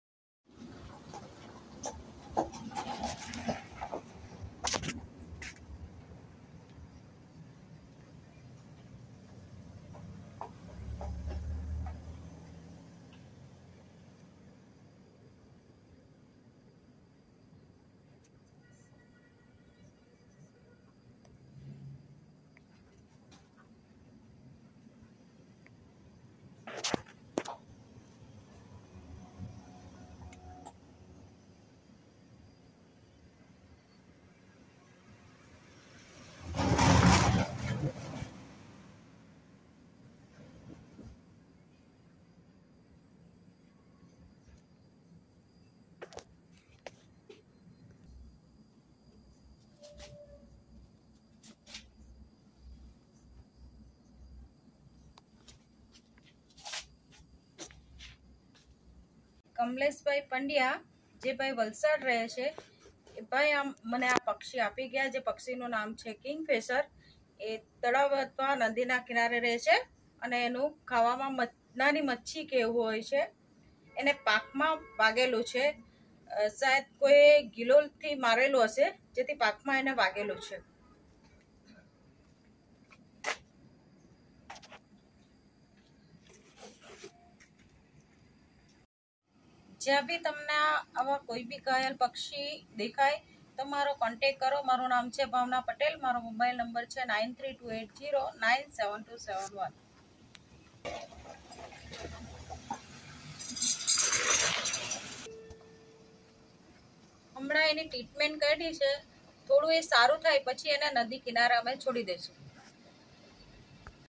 गिलोल से घायल हुवा पानी मे मछली खाने वाला किंगफ़िशर...पक्षी...
snake video wildlife animal helpline number bhavna patel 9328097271 snake helpline india